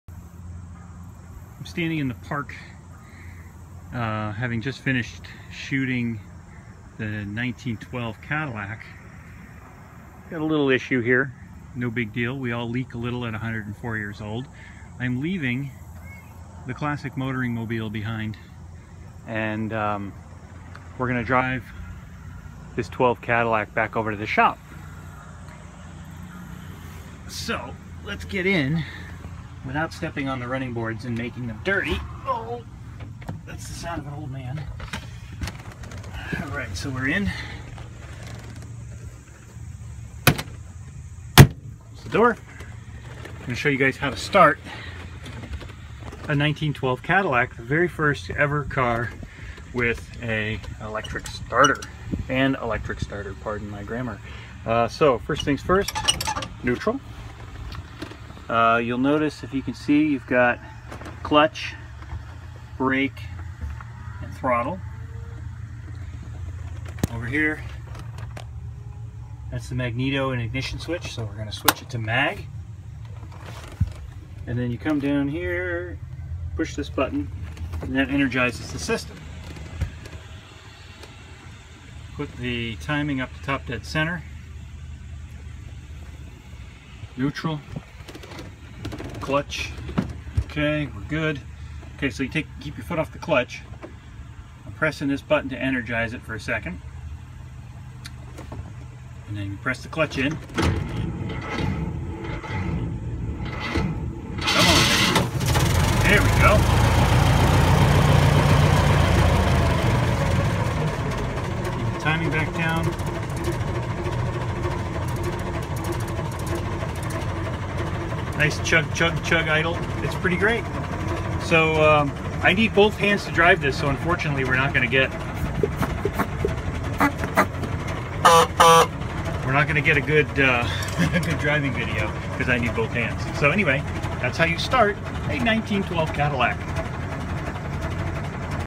1912 Cadillac Self-Starter
How to start a 1912 Cadillac Model 30 Tourer with the Delco Self Starting System.